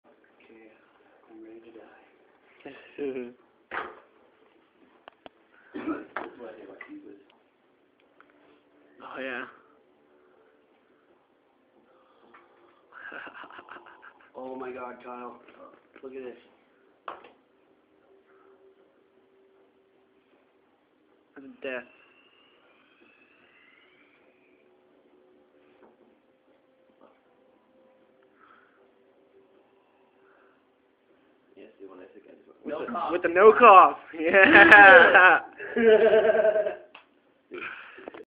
No cough
Zach took this one hit... And survived the "Tearjerker"